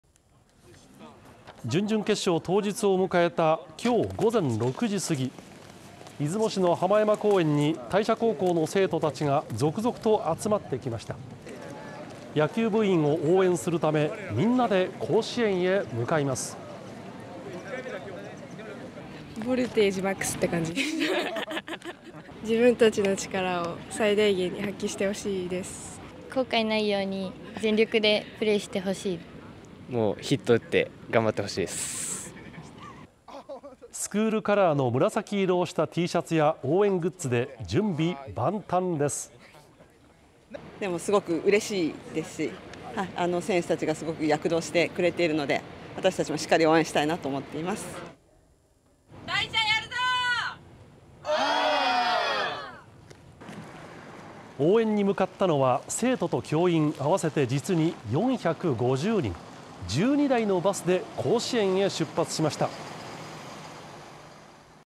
【107年ぶりの4強入りへ】バス12台！大社高校の応援団が甲子園に向かって出発 島根県出雲市
8月19日、107年ぶりの夏の甲子園ベスト4を懸け、準々決勝に臨む島根県代表の大社高校。19日朝、生徒や教員の応援団が甲子園球場に向け出発しました。

8月19日午前6時過ぎ、準々決勝の日を迎えた大社高校野球部を応援しようと、島根県出雲市の浜山公園に、同校の生徒たちが続々と集まっていました。大舞台に臨む野球部に声援を送るため、バスで約３００キロ離れた球場へ向かいます。

大社高校の生徒
「ボルテージMAXって感じ。自分たちの力を最大限に発揮してほしいです」

「後悔のないように、全力でプレーしてほしい」

スクールカラーの紫色をしたTシャツや応援グッズで準備万端です。

大社高校　吉川めぐみ 校長
「すごくうれしいですし、選手たちが躍動してくれているので、私たちもしっかり応援したいと思っています」

「大社やるぞー！オー！」

応援に向かったのは、生徒と教員合わせて450人。12台のバスで甲子園球場へ出発しました。